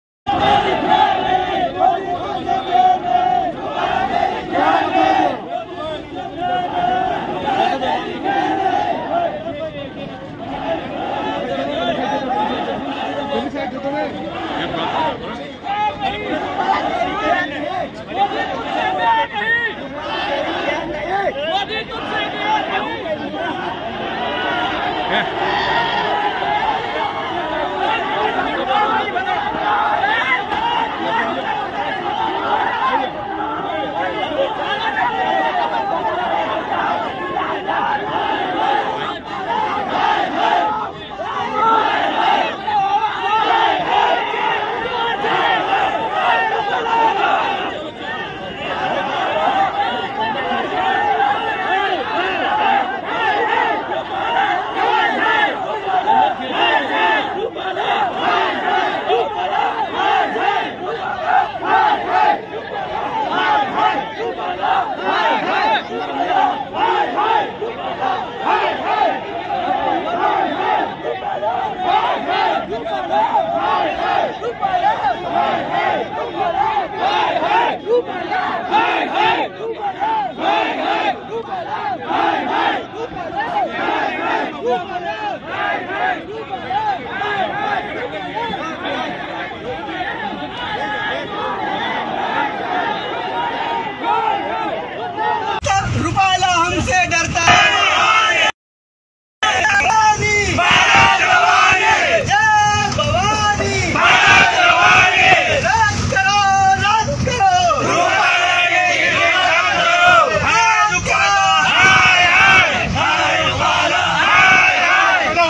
અલીપુરા ચોકડી પર જબરજસ્ત વિરોધ પ્રદર્શન,સૂત્રોચ્ચાર. પોલીસે અનેક ડિટેઇન કર્યા,?
MPAssembly
Orsang Sandesh
National Plus
Gujarat first
bodeilly live
Teer Samachar
Navi teamliavi teamli
Gam Sahi